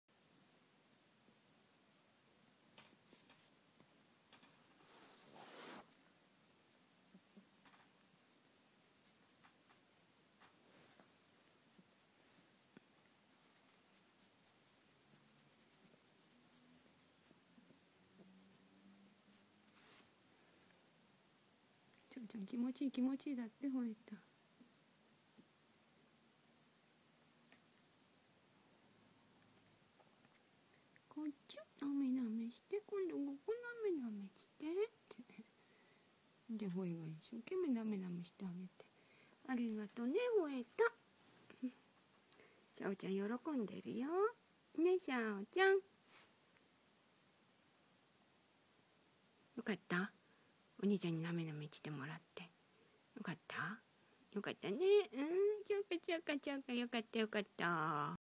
秋の夜長に
ある秋の夜のほえとしゃお。
ほほ寄せ合ったりなめあったり。